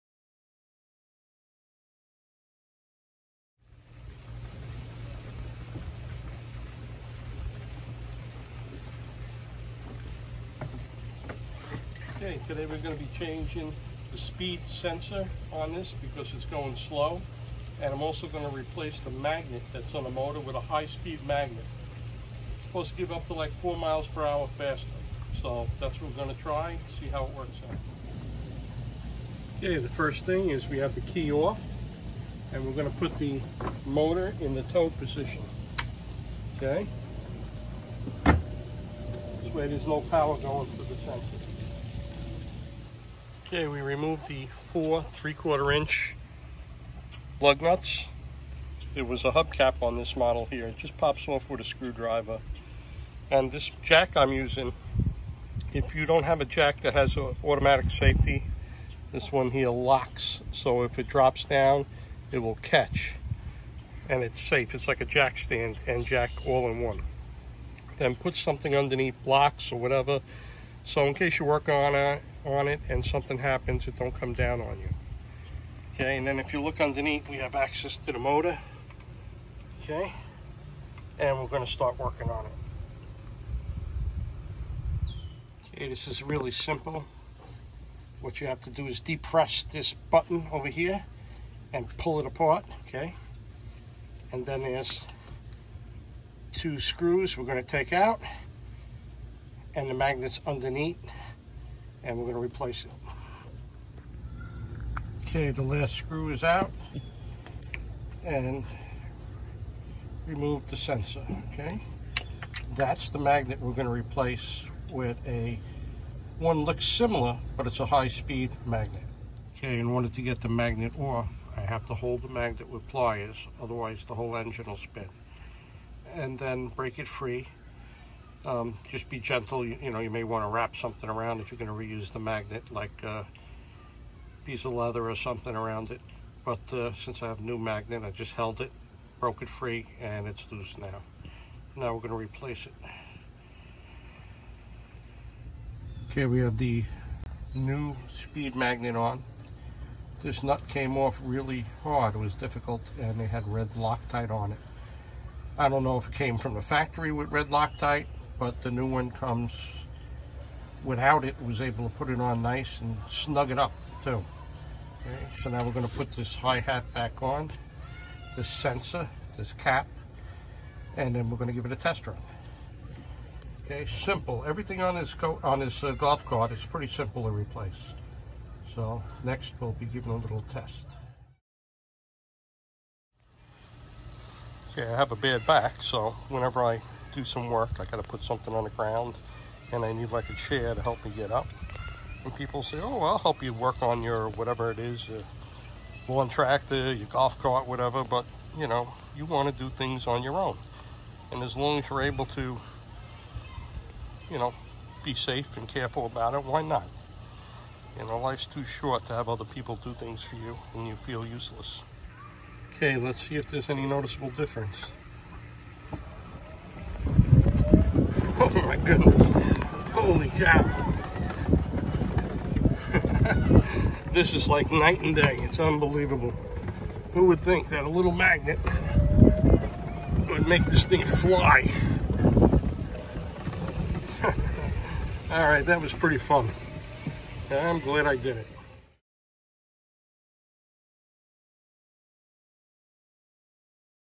Golf cart repair. Club Car speed sensor replacement and high speed magnet.
Club Car speed sensor replacement and high speed magnet.